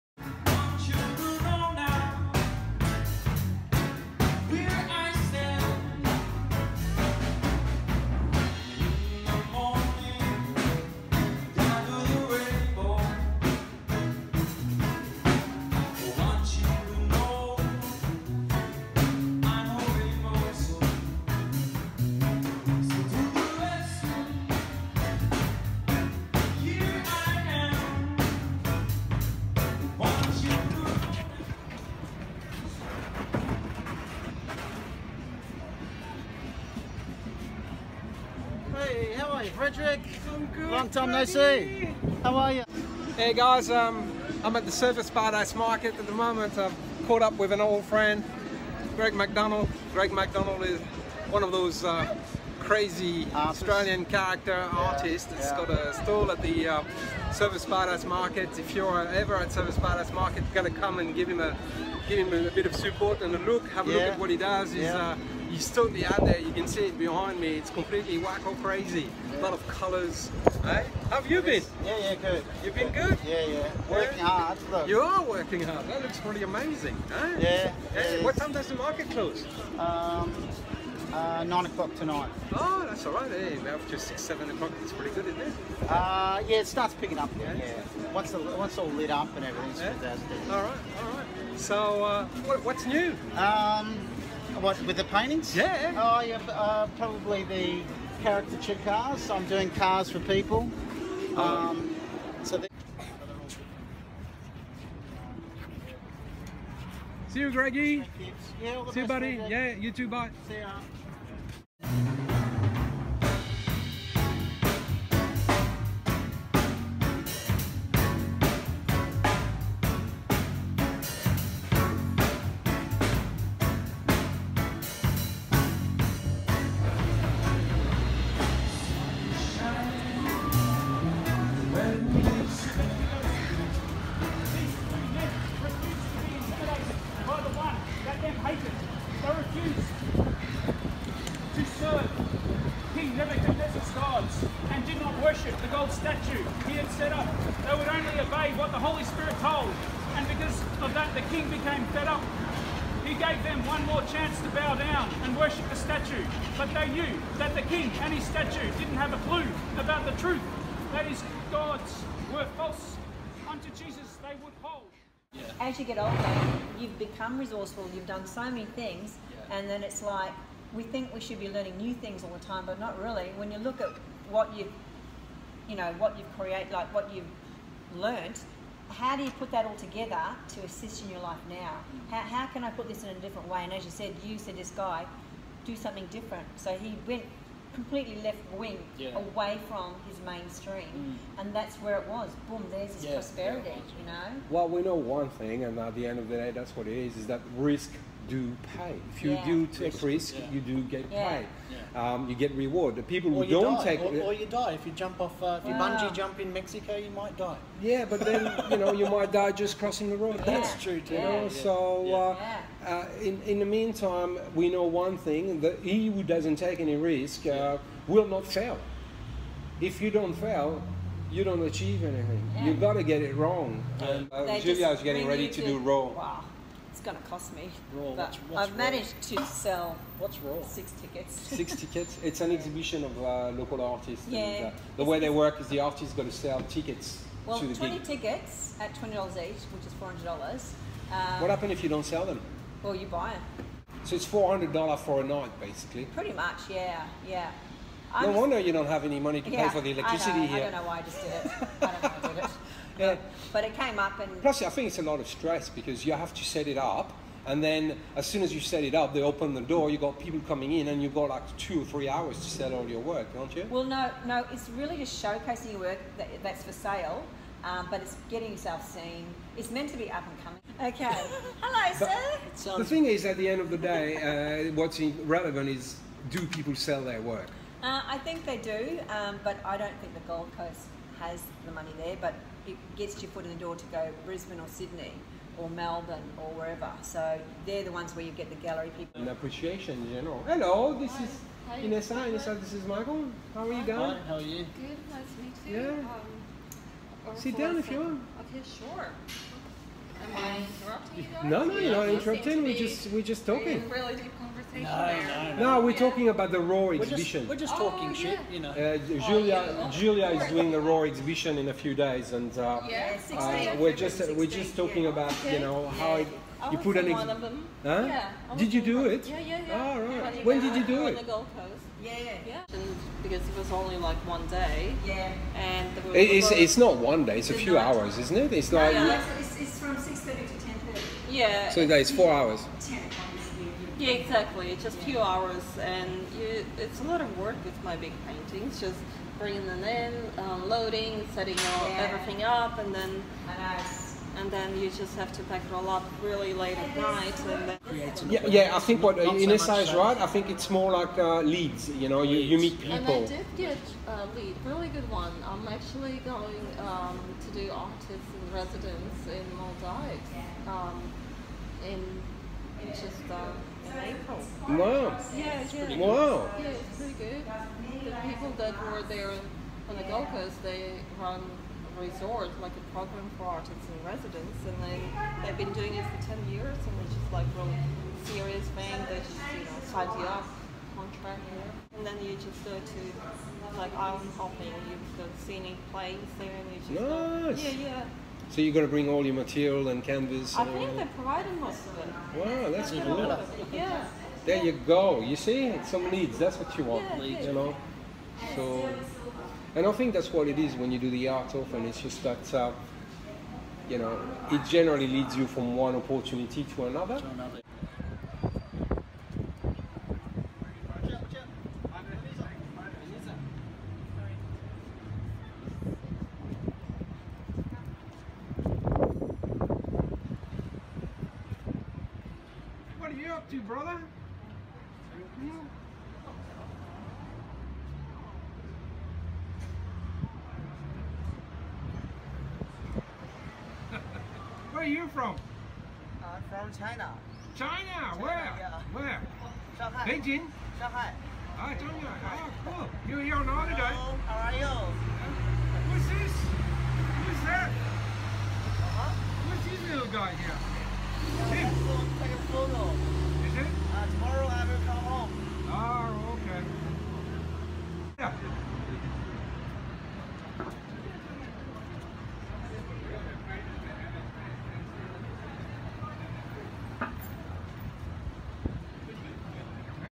Frederic Berjot at Urban Paradise Gallery Gold Coast Surfers Paradise
Frederic Berjot at Urban Paradise Gallery Gold Coast Vlog . Today Urban Paradise gallery artists talk about art and artists opportunity on the Gold Coast
 Surfers Paradise . Gold  Coast